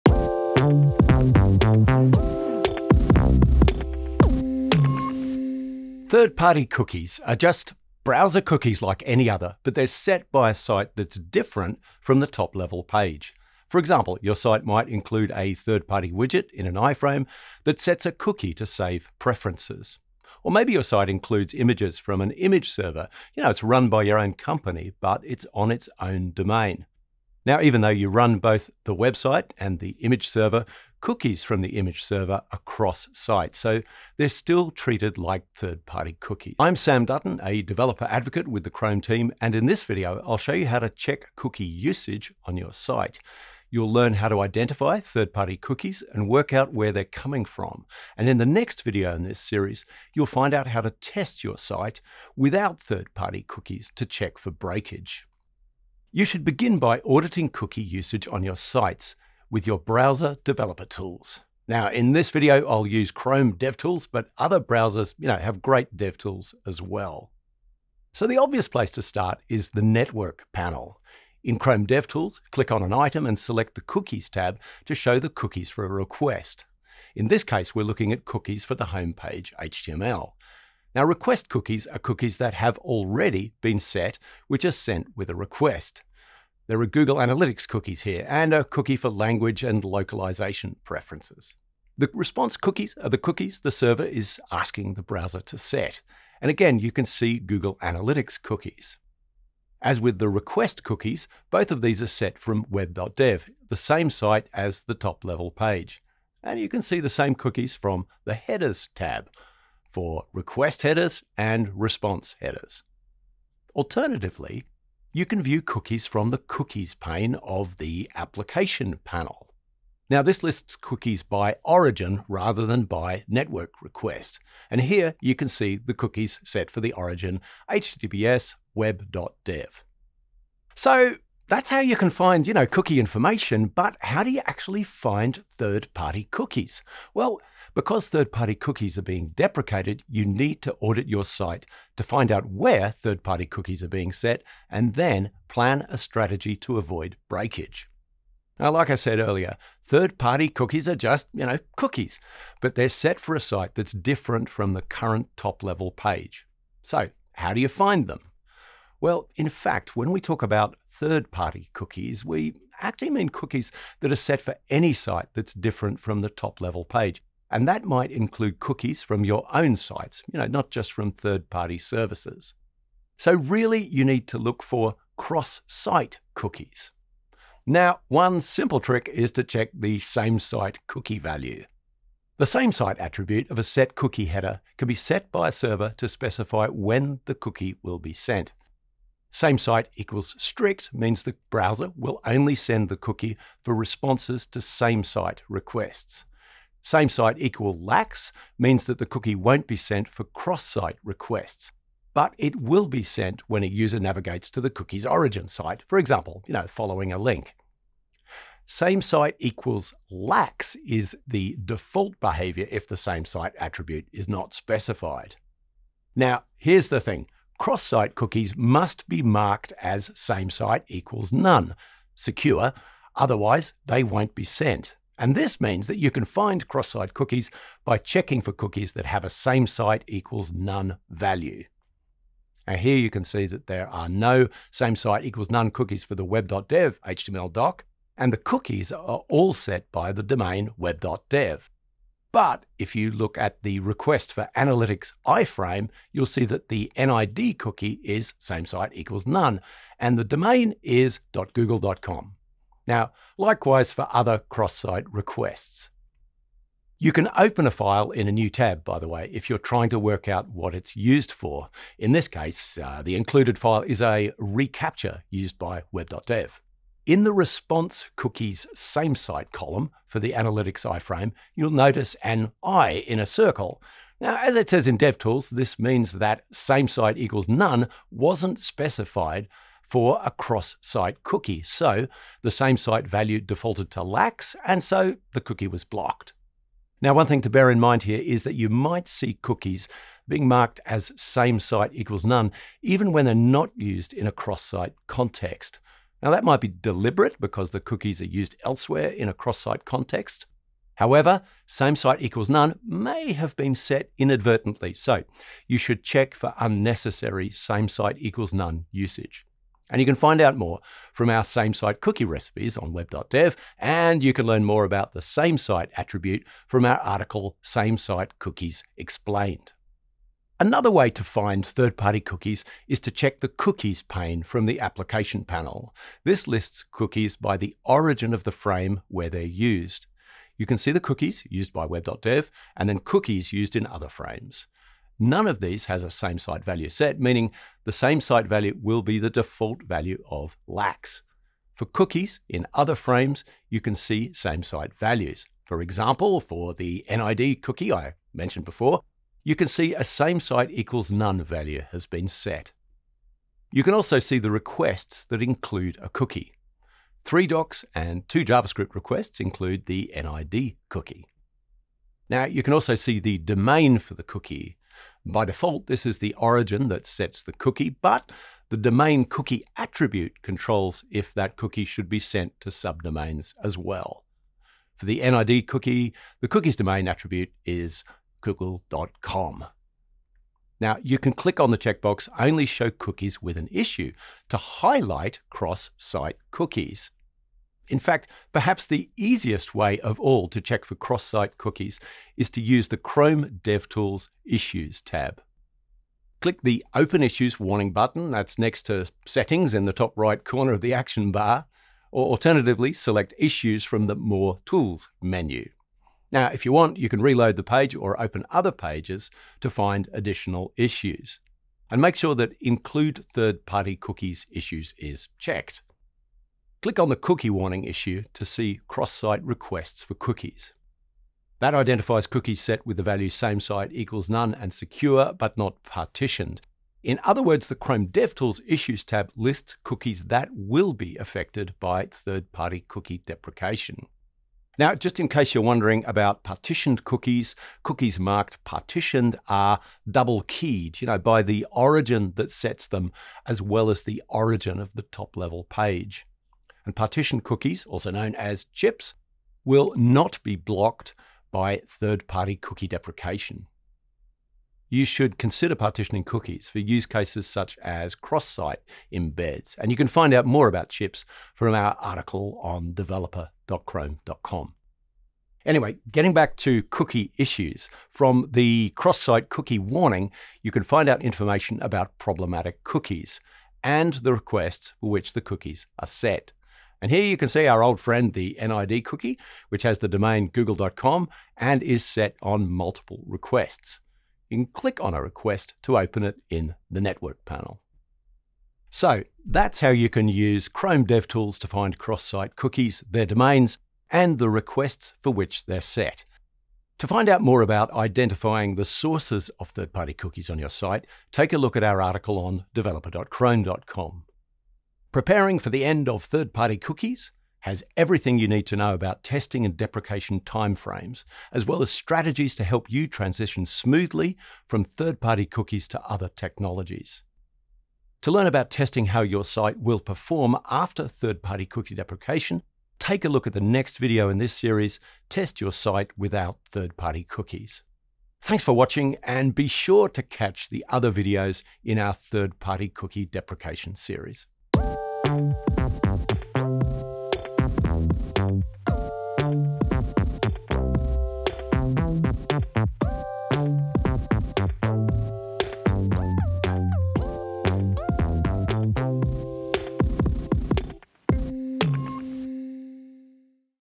Audit your site for third-party cookies
You need to ensure your site works well whether or not third-party cookies are available. In this video we'll show you how to check cookie usage on your site. You'll learn how to identify third-party cookies, and work out where they're coming from.

00:00 Intro
01:04 View cookie information in Chrome DevTools
02:19 Use the DevTools Network panel to identify third-party cookies
05:28 Use the DevTools Applications panel to list cookies by origin
06:36 Use the DevTools Issues tab to identify cookie issues
08:16 Find out more